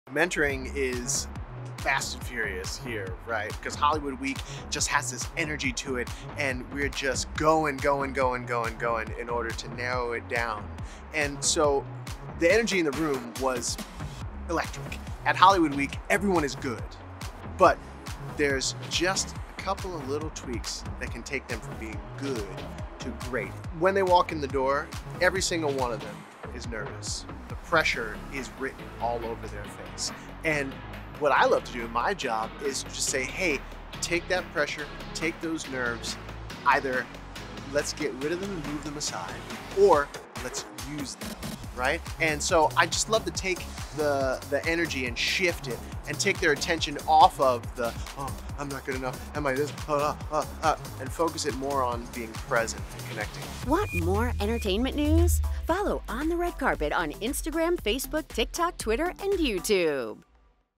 Interview with "American Idol" alum Justin Guarini
All the contestants are good during Hollywood Week at "American Idol." Season 1 finalist and current mentor Justin Guarini says it's his job to help make them better. Watch americanidol Sundays at 8p|7c on ABC